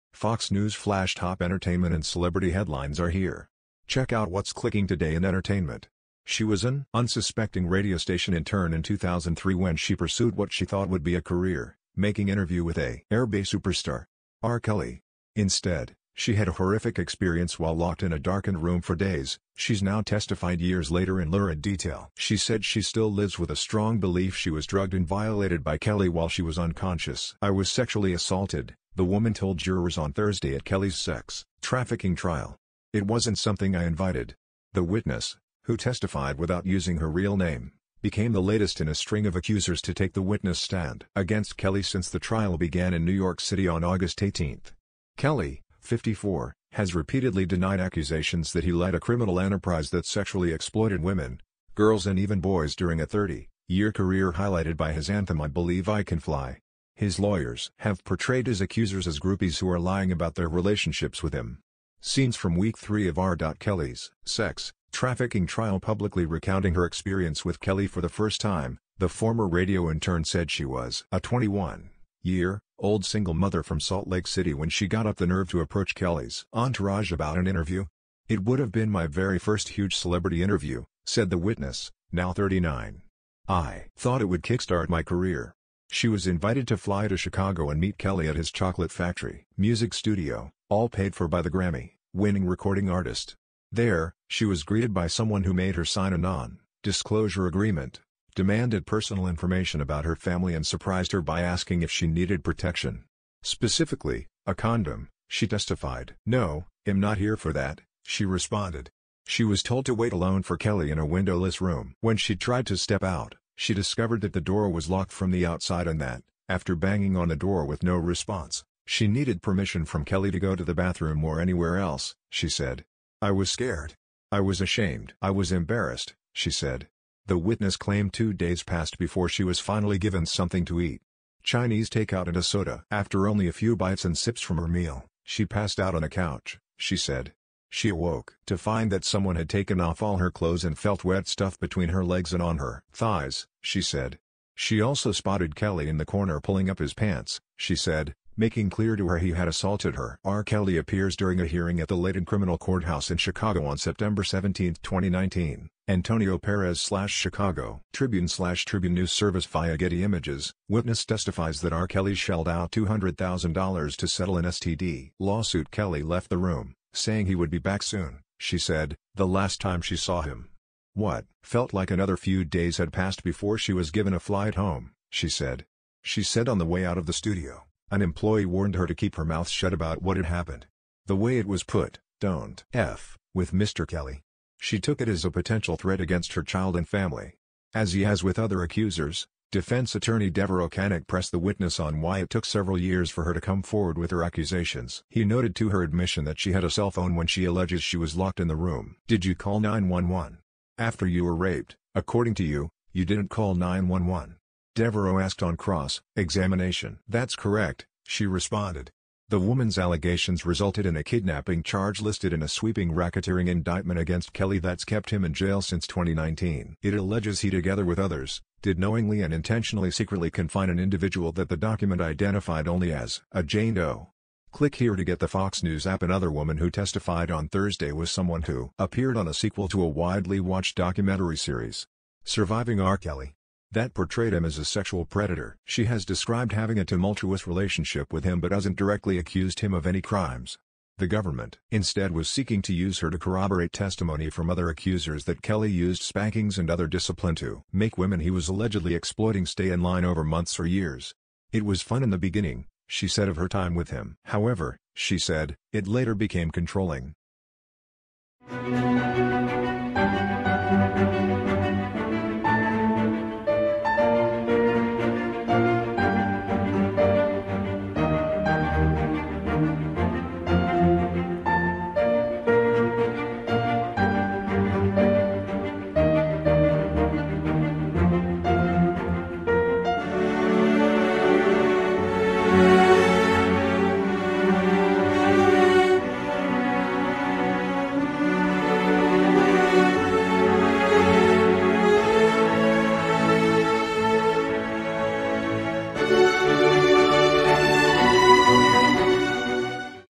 Witness says R. Kelly kept her locked up before sex assault
Fox News Flash top entertainment and celebrity headlines are here. Check out what's clicking today i...